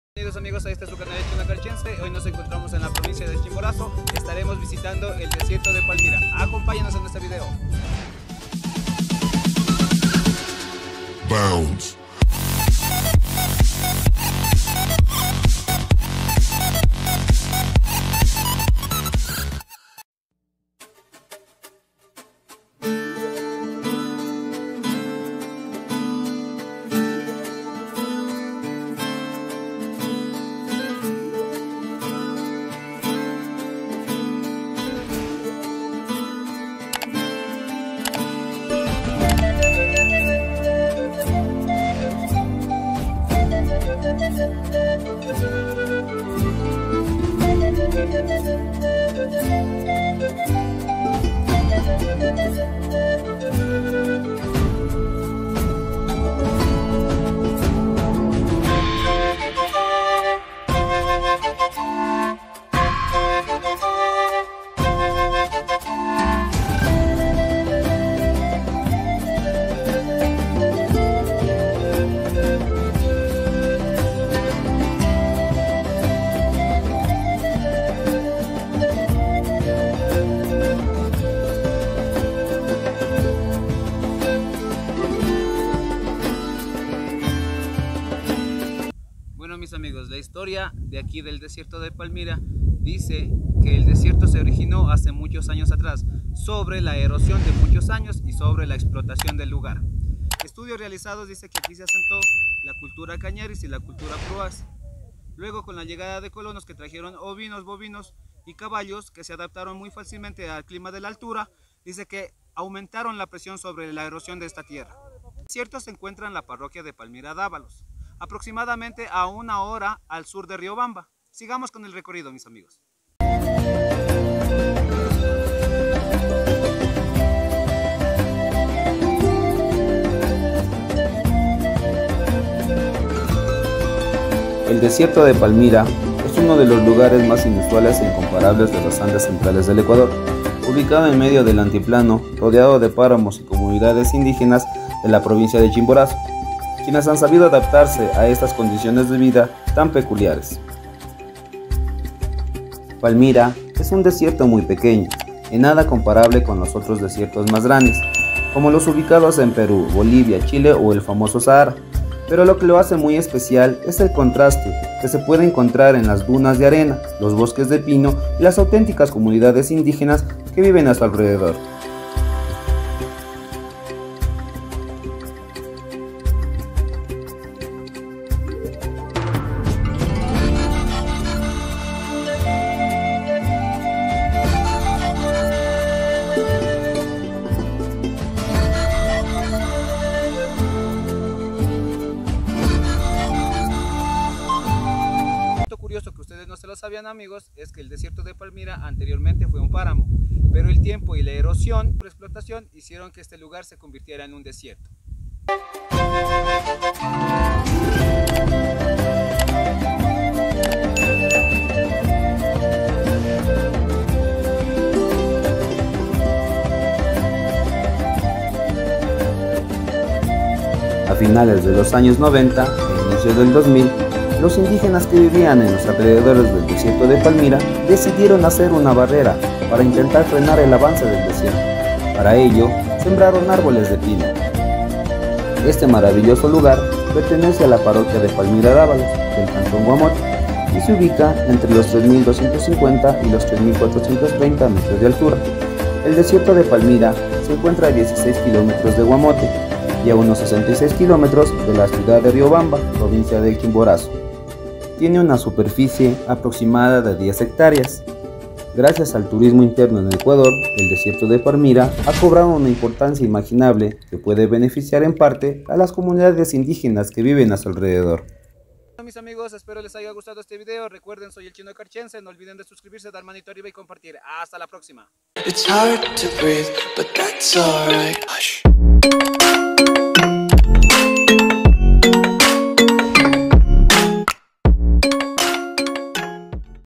VISITAMOS UN DESIERTO EN CHIMBORAZO (DESIERTO DE PALMIRA)🚍🌞🐫| Chino Carchense
AUNQUE NO LO CREAS EXITE UN DESIERTO EN ECUADOR TE CONTAMOS SU HISTORIA DE COMO SE ORIGINÓ Y DONDE QUEDA UBICADO AL IGUAL EXSISTE UN DE PORTE EL CUAL PRACTICAN AVERIGUALO